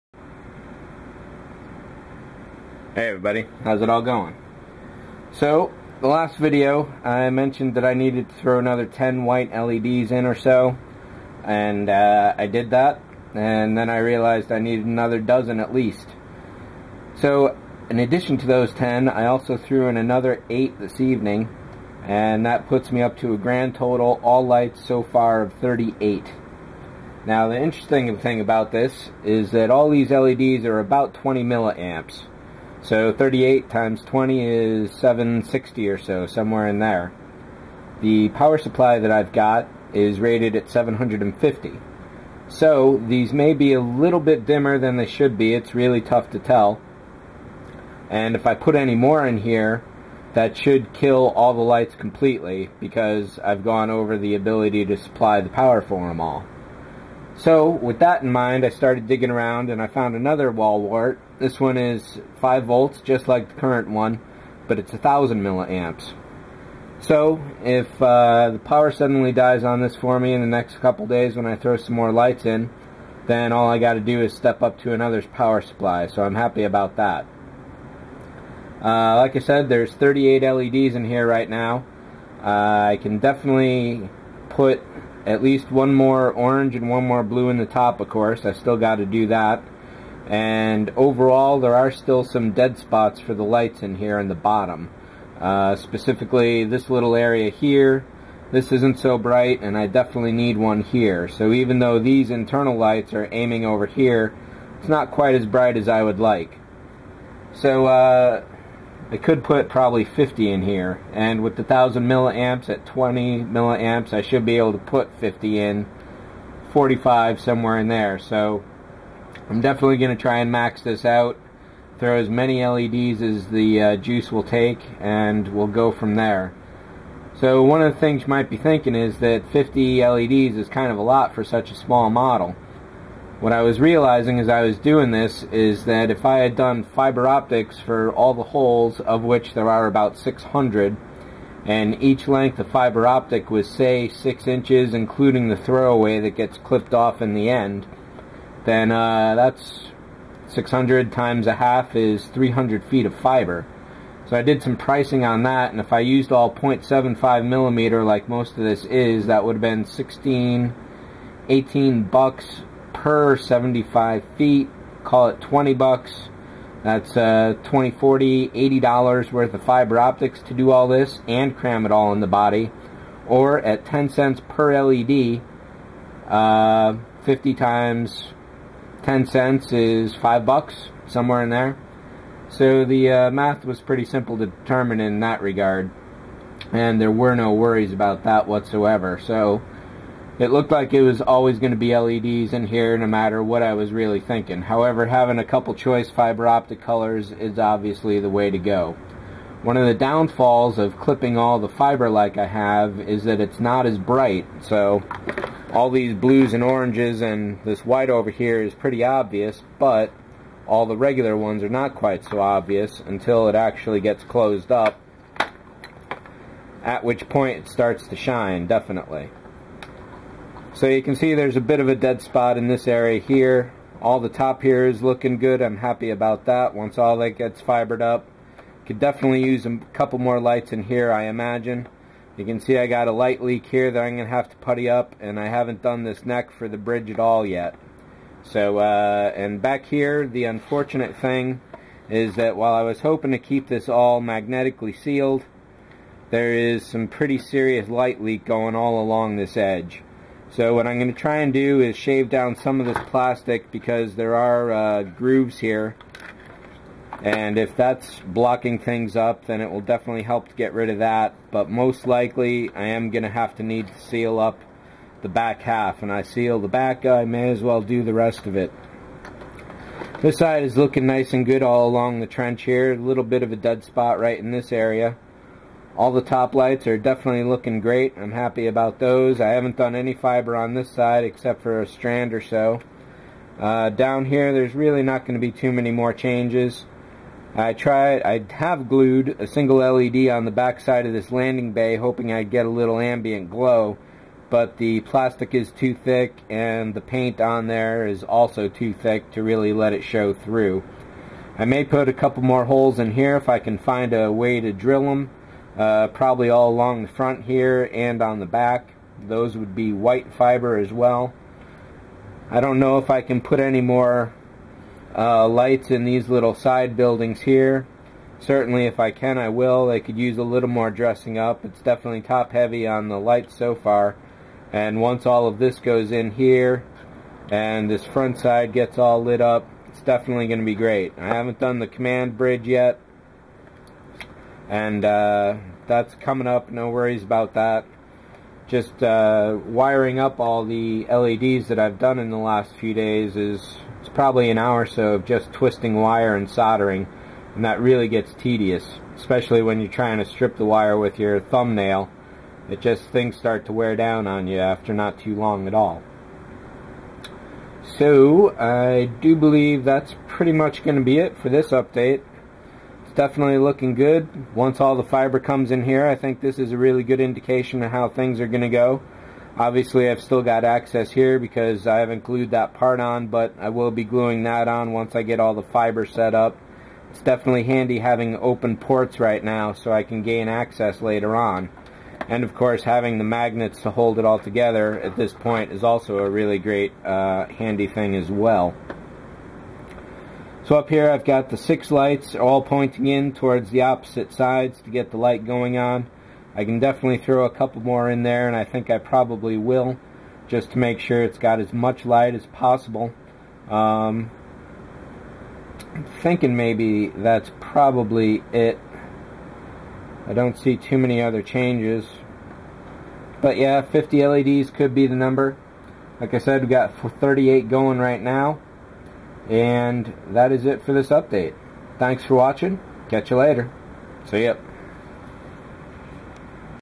MPC Avenger Pt11 More freakin' LEDS?!?
The MPC Star Destroyer.  Follow along as I build the kit straight up -with fiber optics!